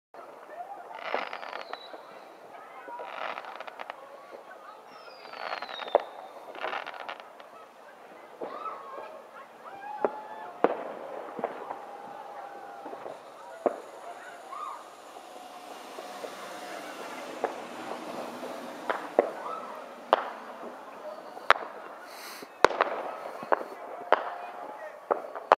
Clonsilla in the snow at New Years
New Years Eve, 2009 into 2010. Ringing in the new year with snow and fireworks.